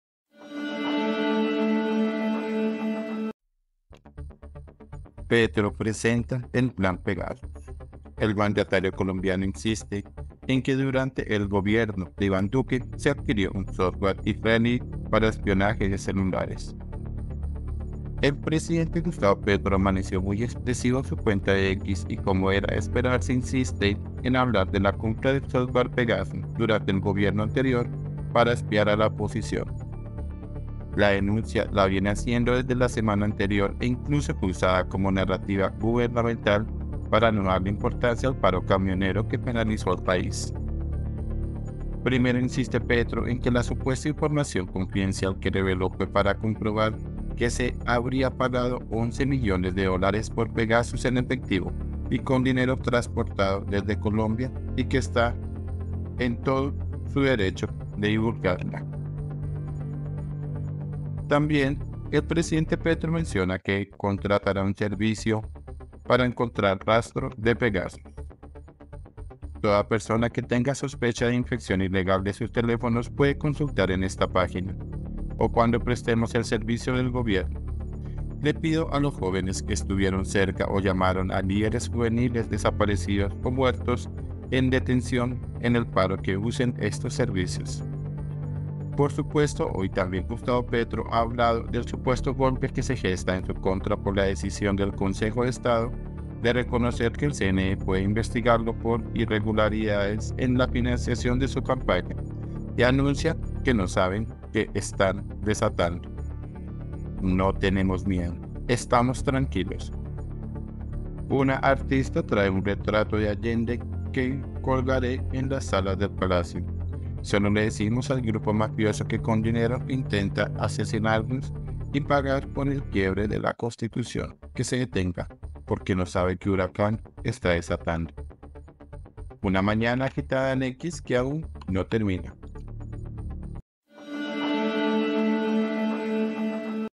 ⛔ PETRO PRESENTA EL 'PLAN PEGASUS'
El mandatario colombiano insiste en que durante el gobierno de Iván Duque se adquirió un software israelí para espionaje de celulares
REVISTA ALTERNATIVA